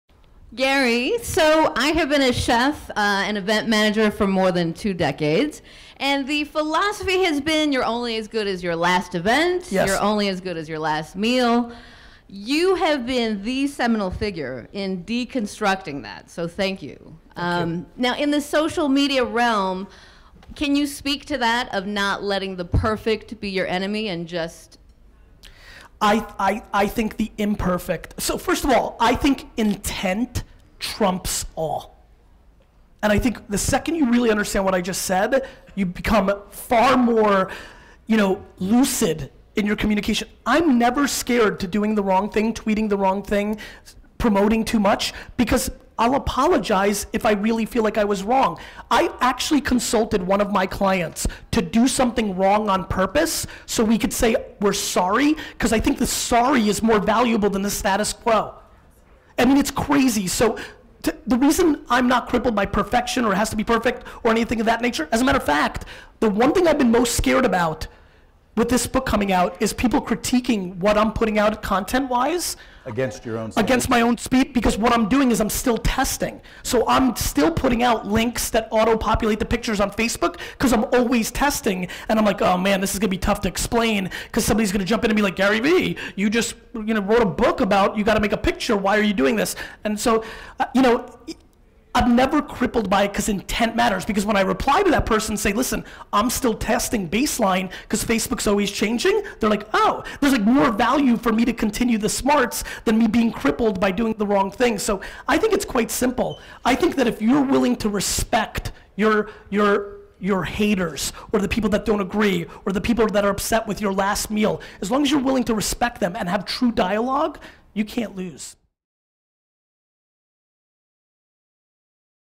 Don't be Crippled by Perfection!
If your intention is pure then there is nothing wrong with apologizing.

How to strive for perfection without being crippled by it! “Gary so I have been a chef and event manager for more than two decades and the philosophy has been you're only as good as your last event, you're only as good as your last meal. You have been the seminal figure in deconstructing that so thank you! Now in the social media realm can you speak to that of not letting perfection be your enemy?”

Intent trumps all! I think the second you really understand what I just said you become far more lucid in your communication. I'm never scared to doing “the wrong thing” of tweeting the wrong thing or promoting too much, because if I know I’m wrong I'll apologize. I actually consulted one of my clients to do something wrong on purpose, so we can say we're sorry because I think the sorry, is more valuable than the status quo.

I'm not crippled by perfection or it has to be perfect or anything in that nature as a matter of fact one thing that I’m most scared about with this book coming out is people critiquing what I'm putting out; content wise against my own speaking, because what I'm doing I’m still testing so I'm still putting out links that auto populate the pictures on Facebook. I am always testing and “I'm like all man this is going to be tough to explain, because someone is gonna jump in and be like Gary Vee you just wrote a book about it why are you doing this?” I'm never crippled by it, intent matters!

When I reply to a person saying listen I'm still testing baseline, because Facebook's always changing and they are like “ohh” and there is more value for me to continue the smarts than me being crippled by doing the wrong thing.

I think it's quite simple, I think that if you're willing to respect your haters or the people that don't agree or the people that are upset with your last meal as long as you're willing to respect them and have true dialogue, you can't lose.